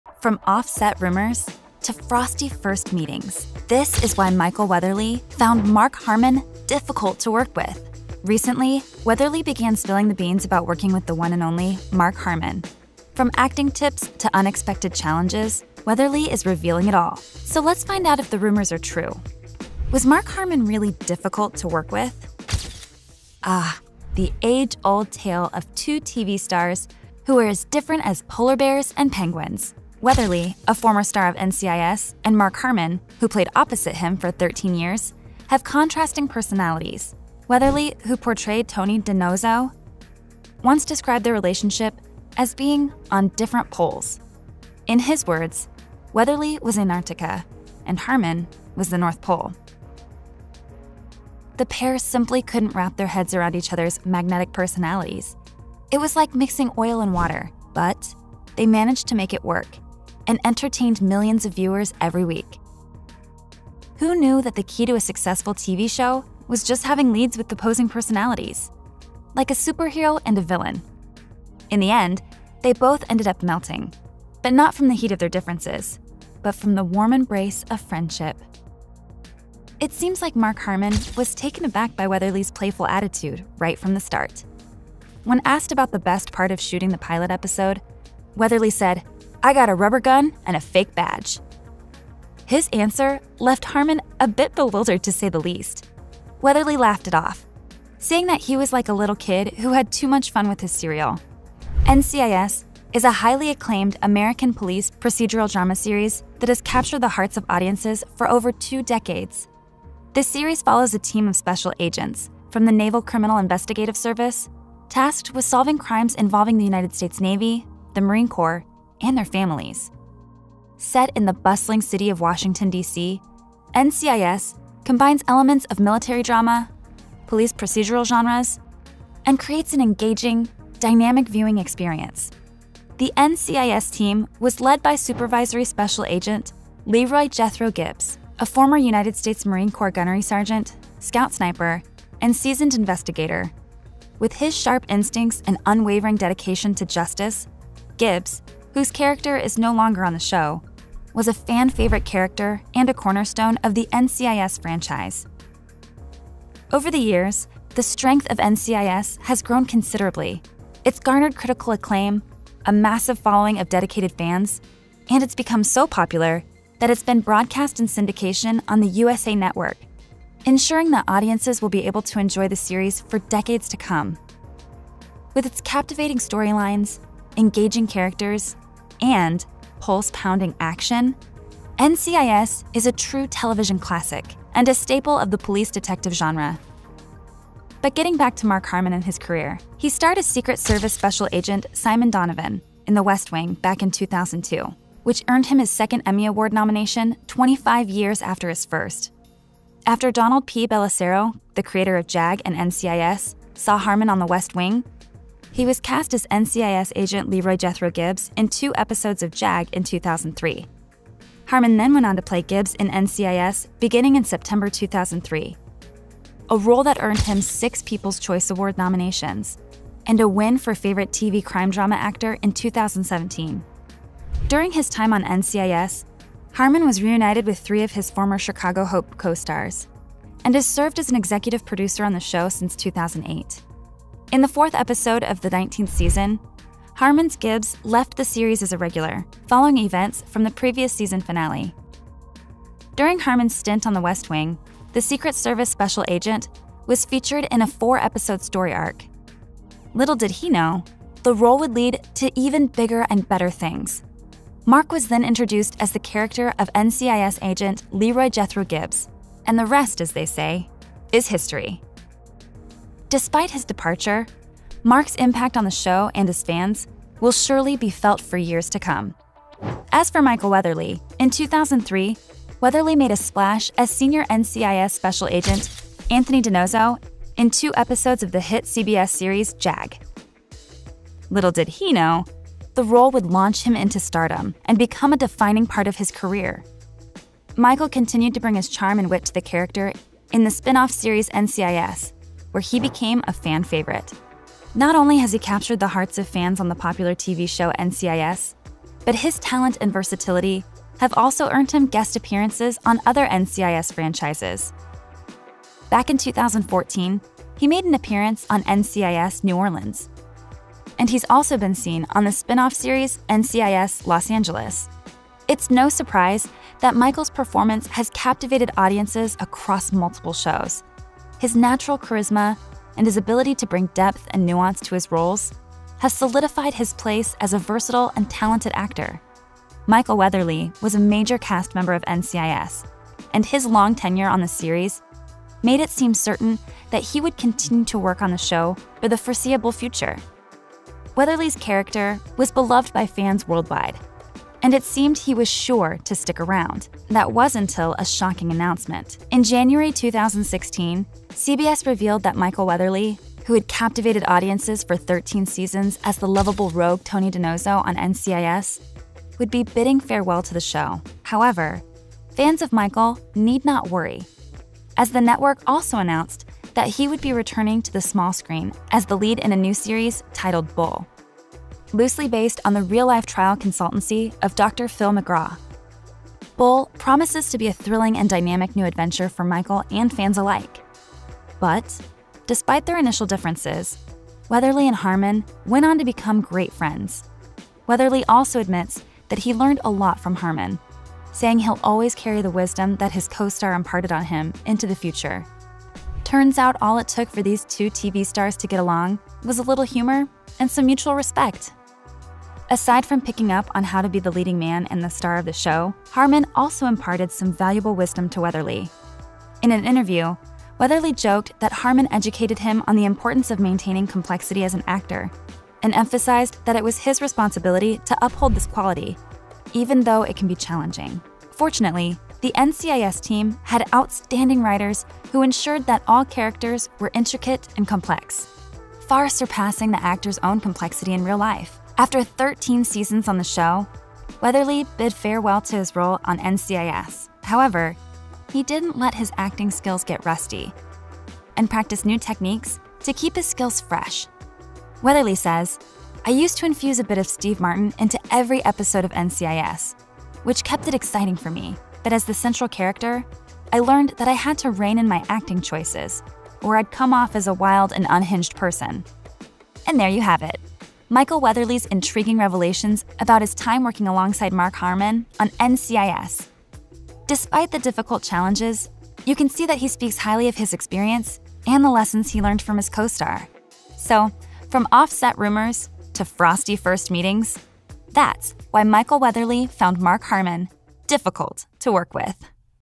Michael Weatherly Speaks On Working With Mark Harmon..
Welcome back to 'Show Pop,  today on the channel we're going to be talking about Michael Weatherly Speaks On Working With Mark Harmon, if you're excited to learn more about this then make sure you watch the whole way through because you don't want to miss the details we have for you!